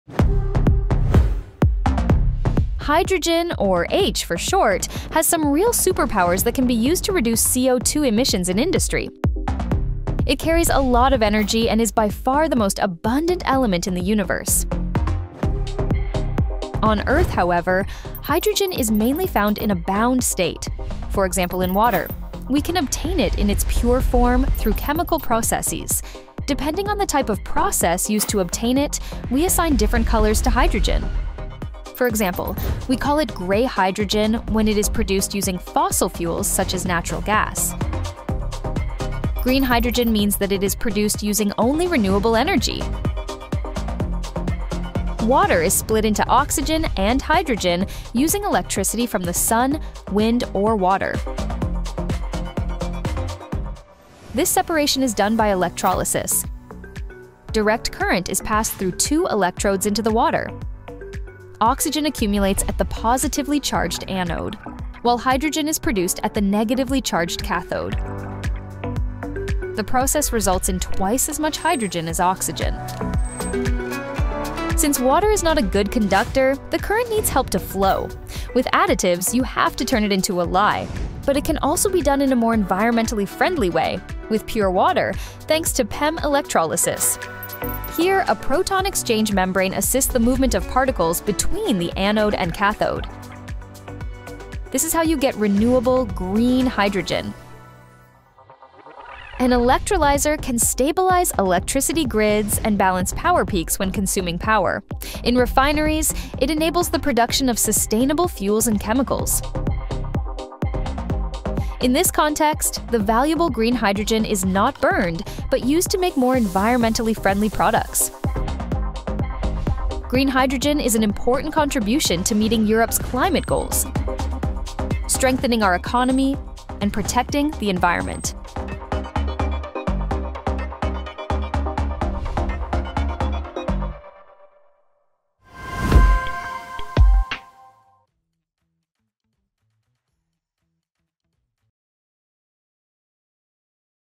Green Hydrogen Explained: Production and Application
Hydrogen, or 'H' for short, has some real superpowers that can help reduce CO2 emissions in industry. To achieve this, we need green hydrogen, which is different from the gray hydrogen produced from fossil fuels. Green hydrogen can be produced in a highly advanced and eco-friendly way using PEM electrolysis. Here, a proton exchange membrane assists the movement of particles between the anode and cathode. In this video, we'll show you how it works and explore the additional benefits of renewable green hydrogen. Dive in and discover more!